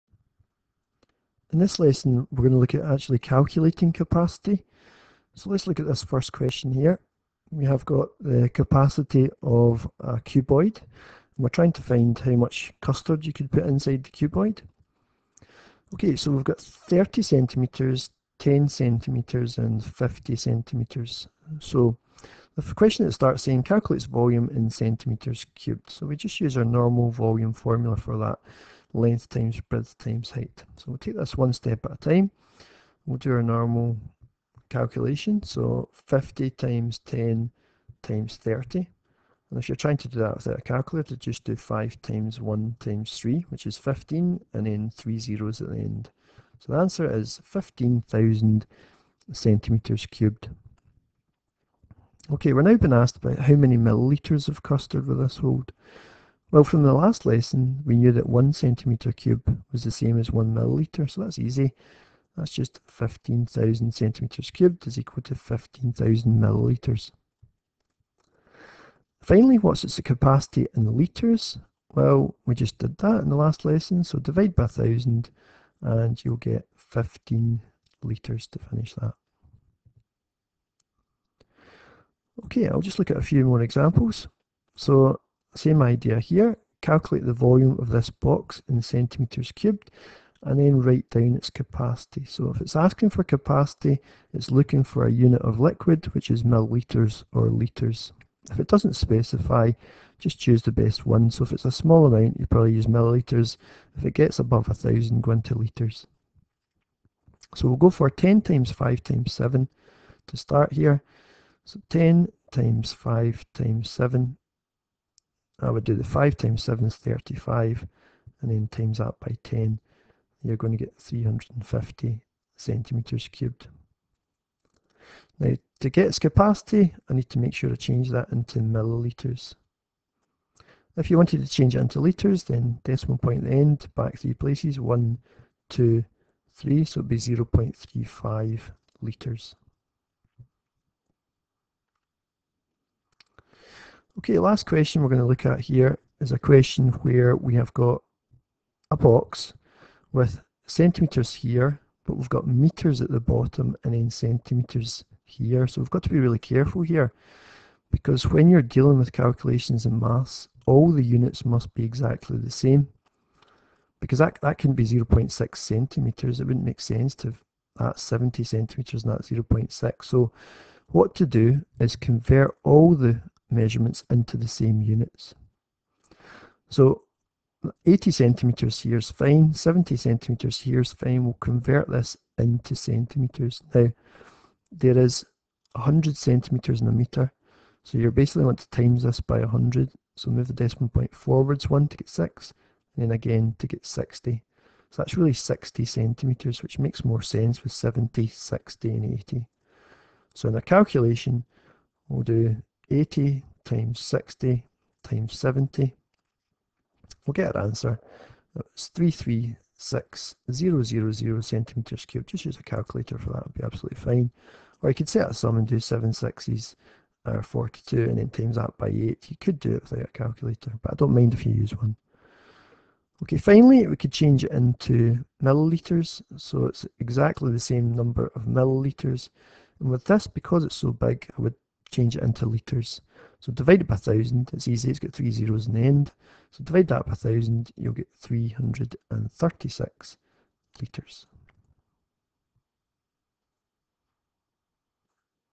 Volume - How to calculate Capacity 2
A Maths lesson on how to calculate the capacity of a cuboid